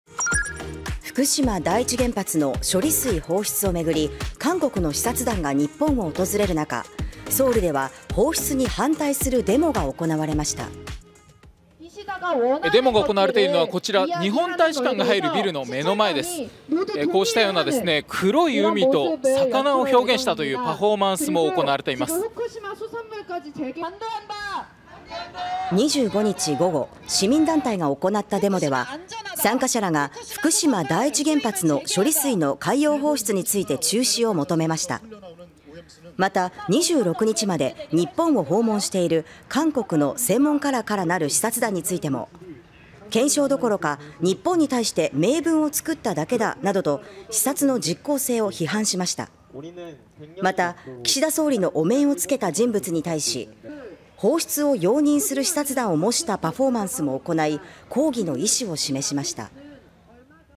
ソウルで福島第一原発の処理水放出に反対デモ 岸田総理模したパフォーマンスで抗議も(2023年5月25日)
福島第一原発の処理水放出を巡り、韓国の視察団が日本を訪れるなか、ソウルでは放出に反対するデモが行われました。

　25日午後に市民団体が行ったデモでは、参加者らが処理水の海洋放出について中止を求めました。

　26日まで日本を訪問している韓国の専門家らからなる視察団についても「検証どころか日本に対して名分を作っただけだ」などと視察の実効性を批判しました。

　また、岸田総理大臣のお面を付けた人物に対して放出を容認する視察団を模したパフォーマンスも行い、抗議の意思を示しました。